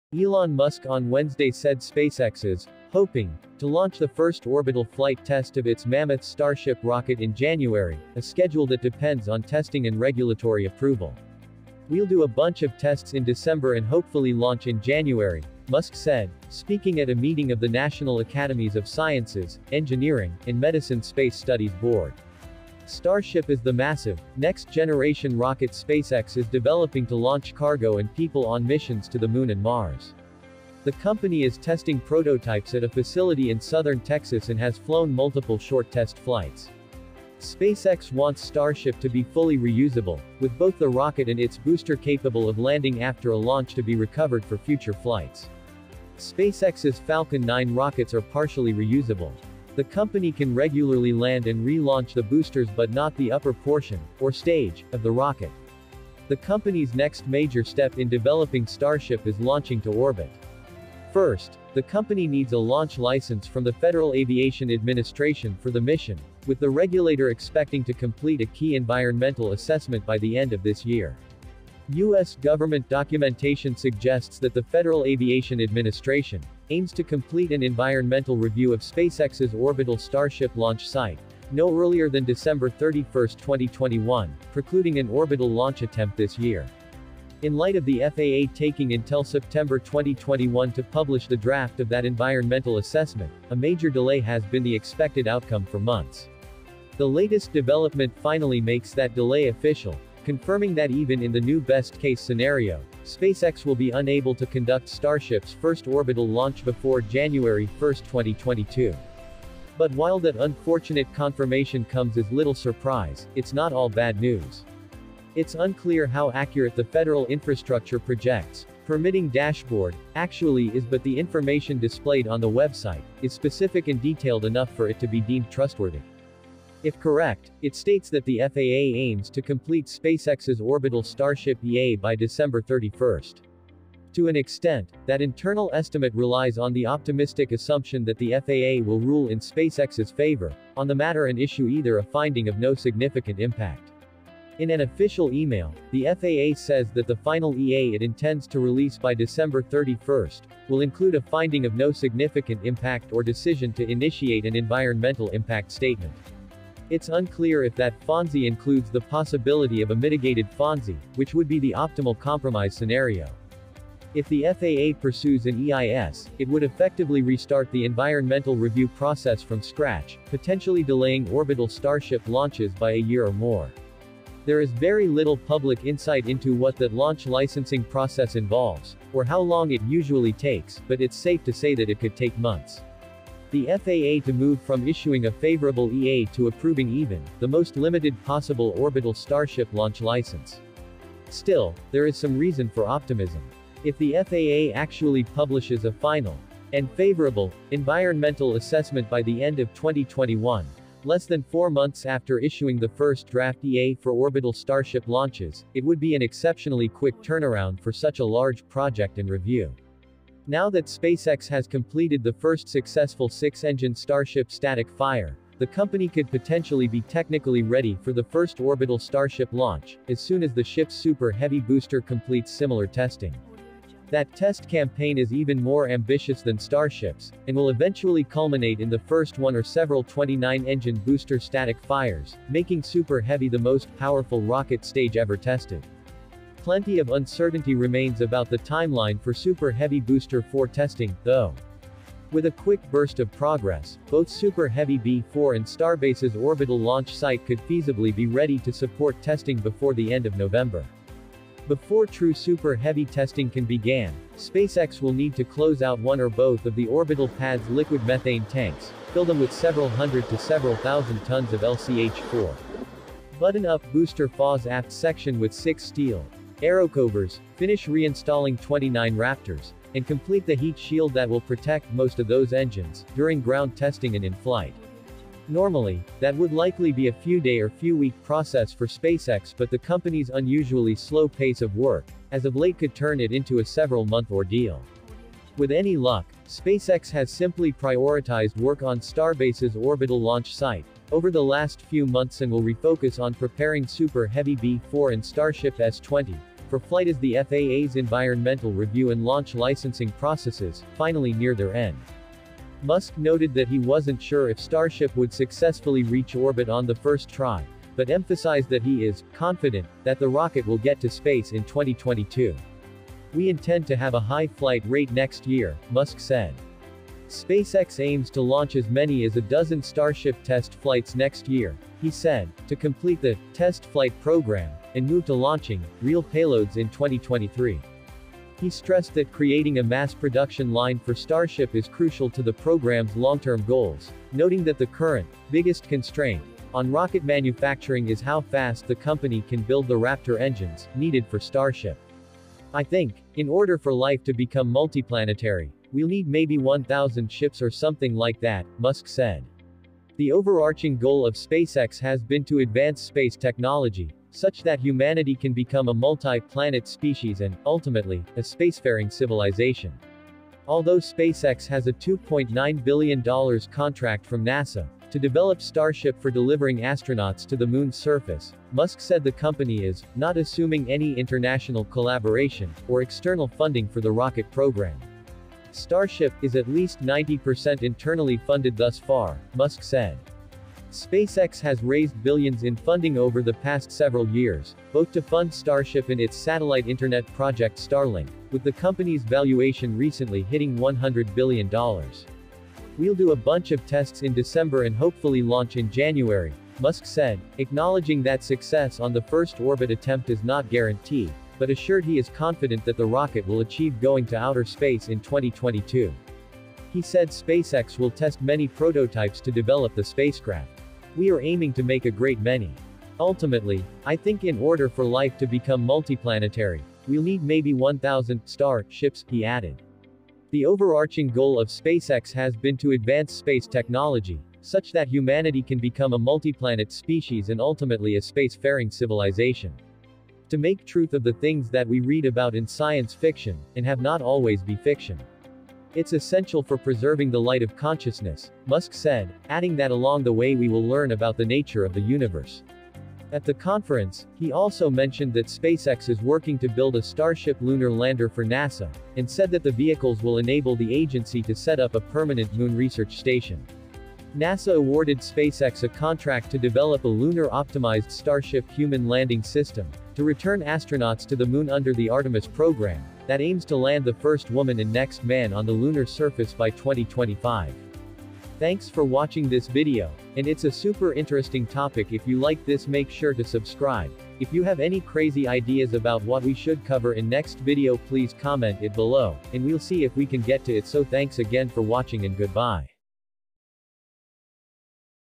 Elon Musk Just OFFICIALLY REVEALED SpaceX First Orbital Launch Flight.
ElonMusk on Wednesday said SpaceX is “hoping” to launch the first orbital flight test of its mammoth Starship rocket in January, a schedule that depends on testing and regulatory approval. “We’ll do a bunch of tests in December and hopefully launch in January,” Musk said, speaking at a meeting of the National Academies of Sciences, Engineering, and Medicine Space Studies Board.

Starship is the massive, next-generation rocket SpaceX is developing to launch cargo and people on missions to the moon and Mars. The company is testing prototypes at a facility in southern Texas and has flown multiple short test flights.

SpaceX wants Starship to be fully reusable, with both the rocket and its booster capable of landing after a launch to be recovered for future flights. SpaceX’s Falcon 9 rockets are partially reusable. The company can regularly land and re-launch the boosters but not the upper portion, or stage, of the rocket.

The company’s next major step in developing Starship is launching to orbit. First, the company needs a launch license from the Federal Aviation Administration for the mission, with the regulator expecting to complete a key environmental assessment by the end of this year.

Followed Youtube Fair Copyright Guidelines.

Credited -
SpaceX